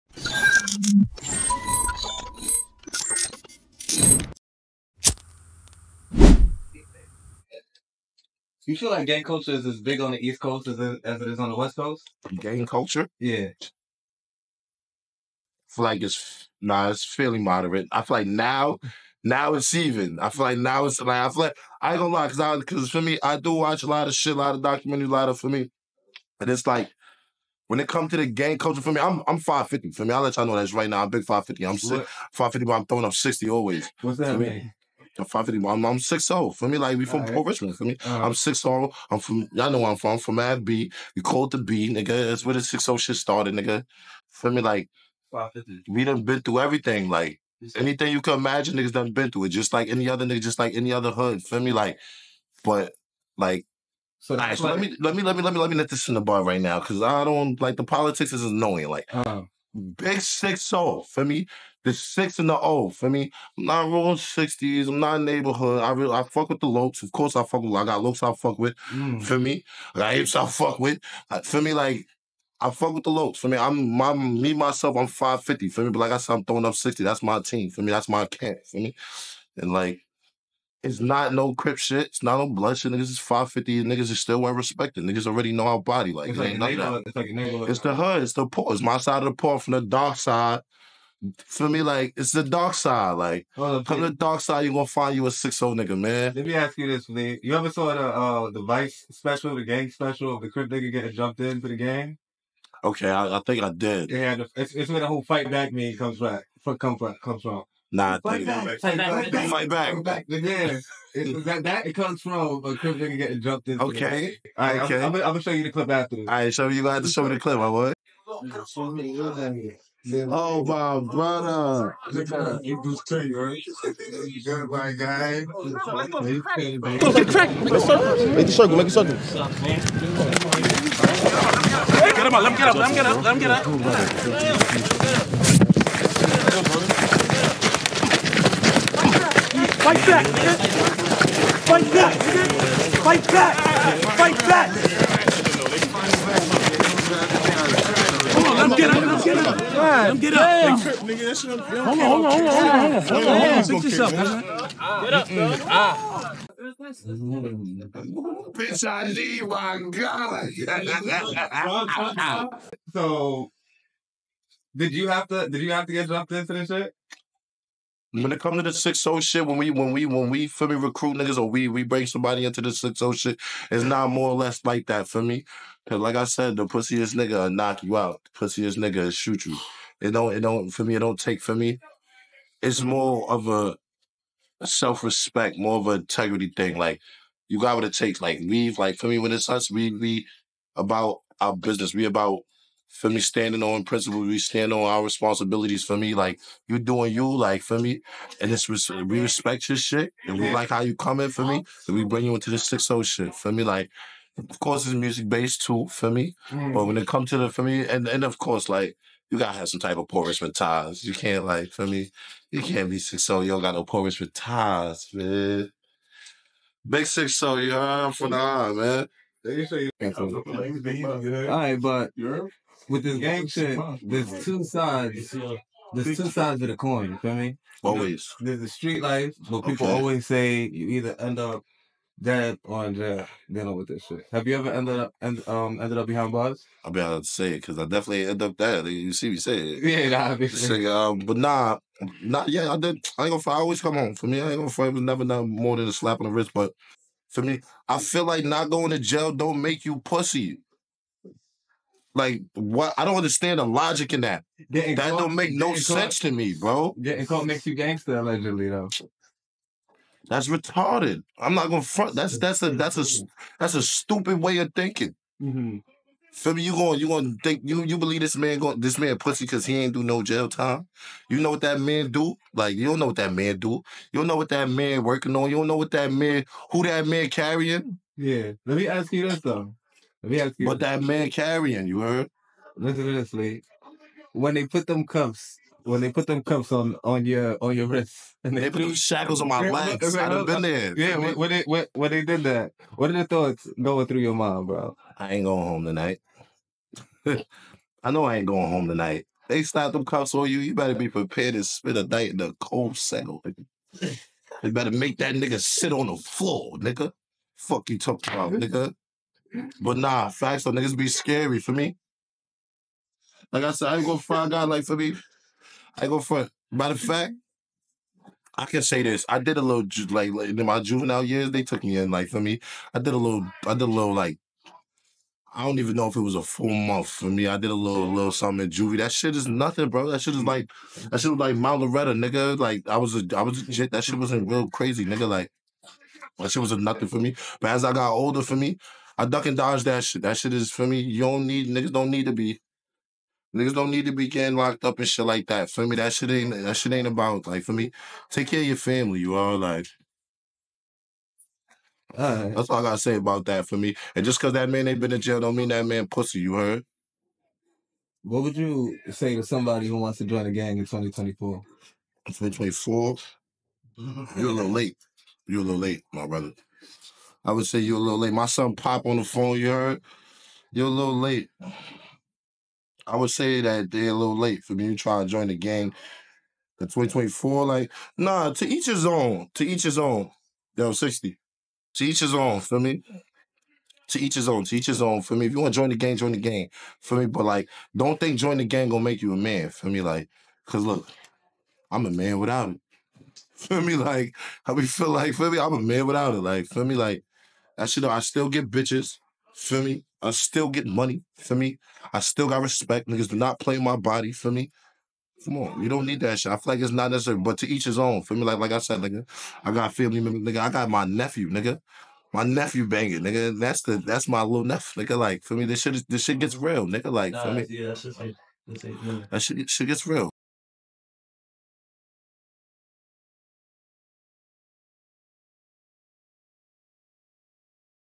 Flee Savage "Im NOT Rolling 60's!", Getting Jumped into Gang & Gang Politics (Part 2)
Get ready to delve deep into the world of gang life as Flee Savage shares his raw and unfiltered experiences, offering insights into the realities of gang politics and the intense process of getting jumped into a gang.

🔫 Street Chronicles:
Join Flee Savage as he pulls back the curtain on the harsh realities of street life. From navigating dangerous territories to understanding the intricate dynamics of gang politics, he offers a no-holds-barred look into the gritty world he calls home.

💥 Gang Politics:
Prepare to be immersed in the complex world of gang politics as Flee Savage provides an insider's perspective on the power struggles and alliances that shape street gangs. With his firsthand knowledge and street smarts, he offers valuable insights into the unwritten rules and codes that govern gang life.

📺 Watch Now:
Get ready for a raw and unfiltered journey into the heart of gang culture with Flee Savage.